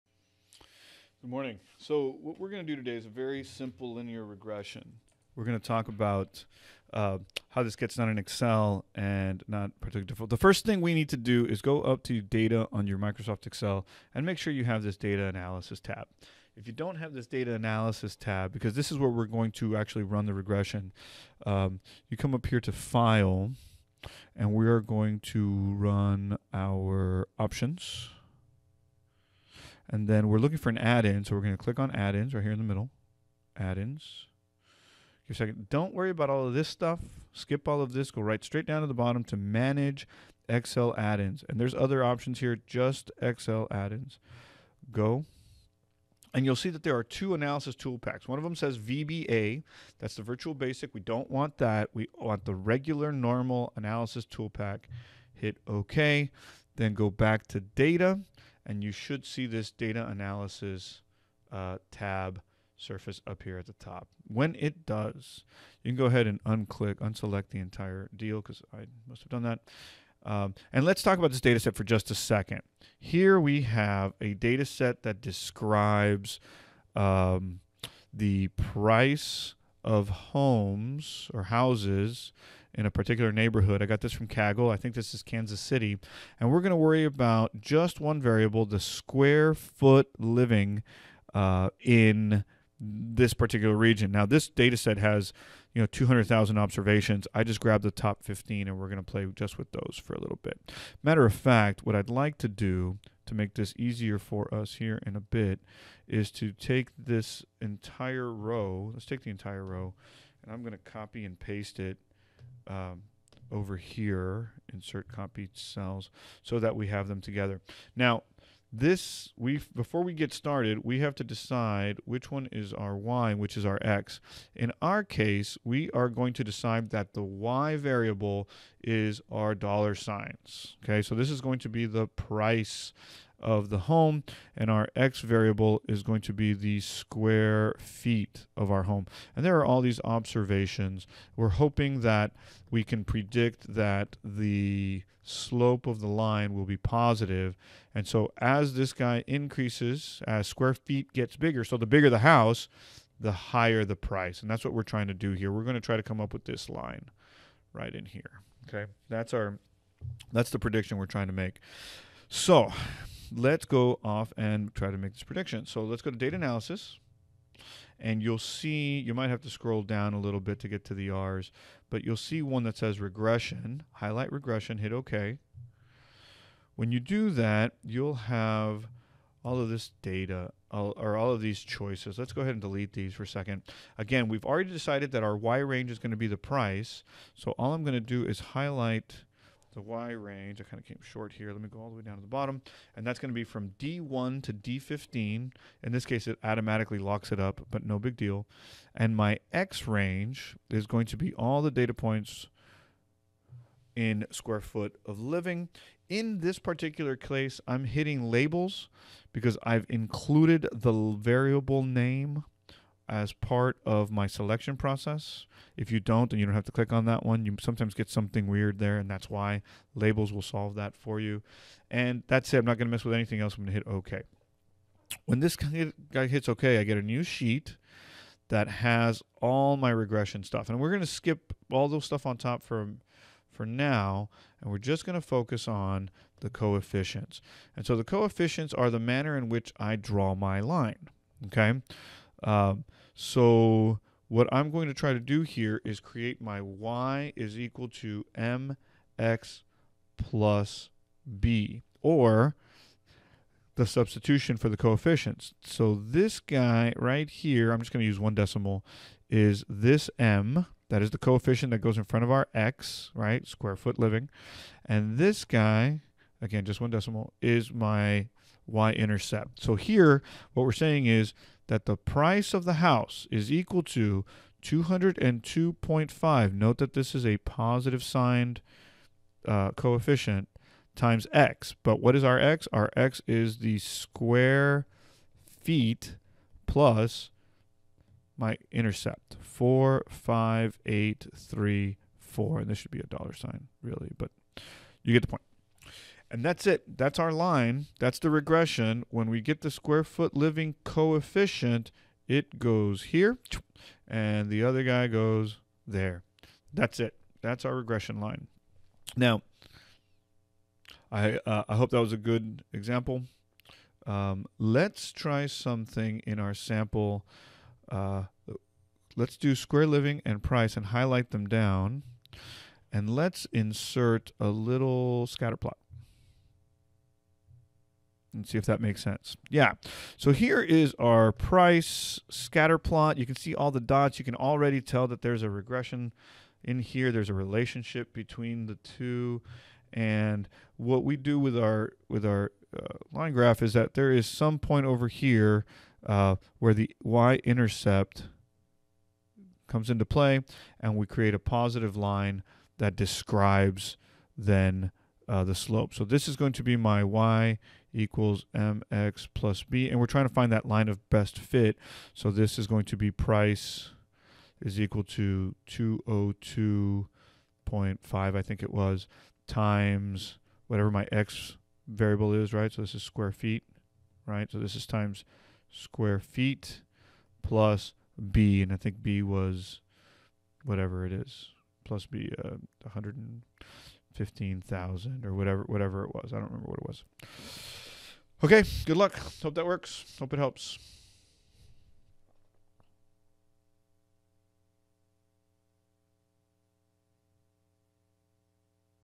statsprof - simple linear regression in excel
If you want to follow along.
It's King's County not Kansas City.  oops. Also, I only used the first 15 or so observations for sake of simplicity.